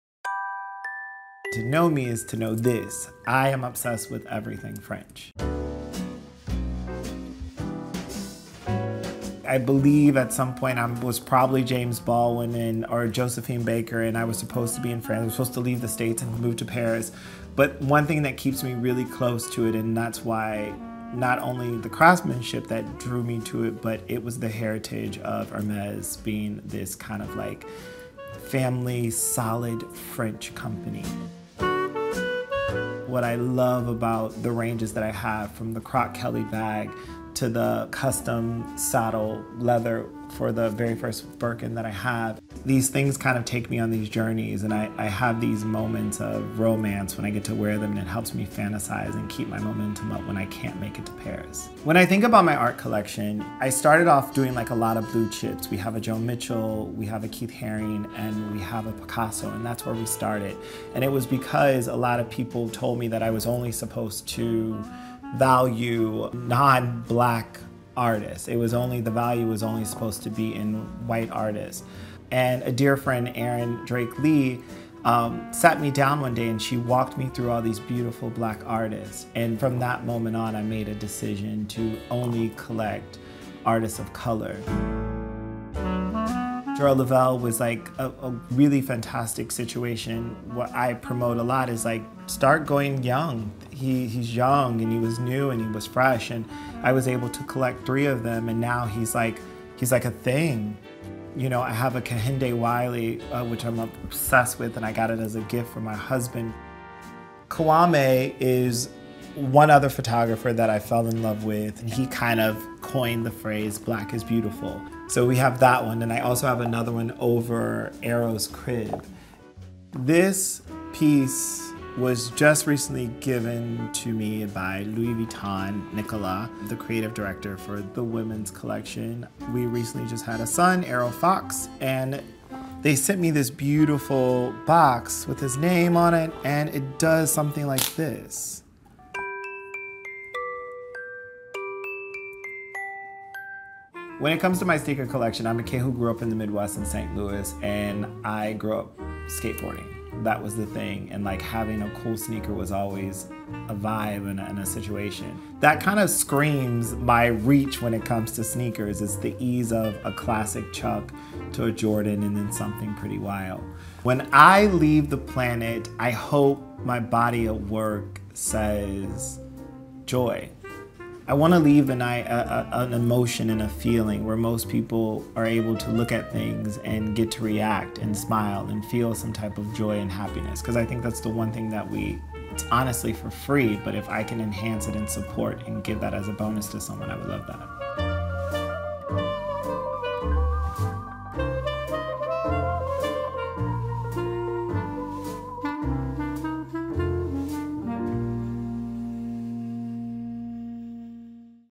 Why I Collect | Episode 3: Jason Bolden, Celebrity Stylist and Creative Director, Contemporary Art a
In this installment of our new series, “Why I Collect,” Sotheby’s heads to Los Angeles to sit down with six collectors from the worlds of film, sports, music, style, and more. Watch as they each share pieces from their collections – ranging from contemporary art to sneakers—and discuss their perspectives on collecting, what got them started, what inspires them, and what they hope their collections say about them.

Meet Jason Bolden, a Los Angeles-based celebrity stylist and creative director.  One of six tastemakers featured in “Why I Collect” – a new series spotlighting young collectors in L.A. who are also driving a surge of creativity in the city – Jason is a collector of contemporary art and luxury objects. His clients include Michael B. Jordan, Cynthia Erivo, Yara Shahidi, Amanda Gorman, Trevor Noah, Janet Mock, Ava DuVerney, and many more.